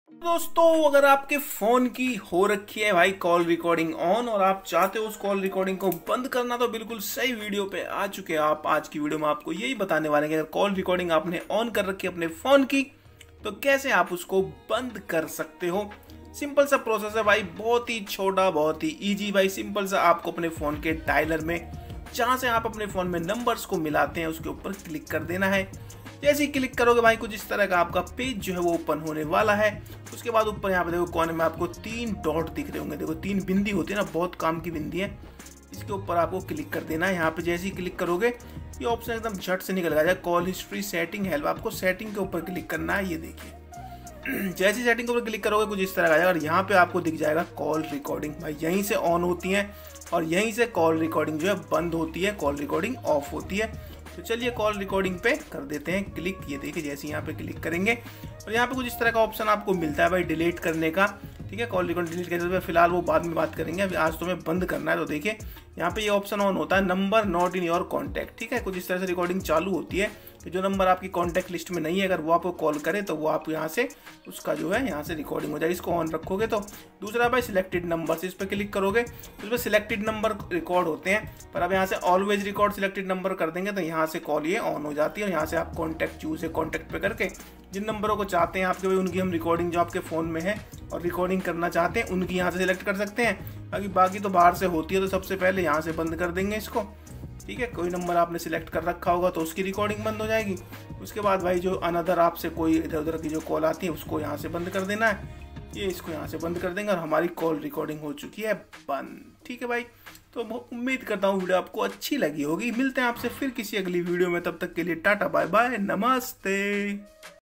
mobile phone mein call recording kaise band Karen
hello friends is video Maine bataya hai ki agar aapke mobile phone mein call recording chalu hai to aap apne mobile phone ki call recording kaise band kar sakte hain video ko pura dekhen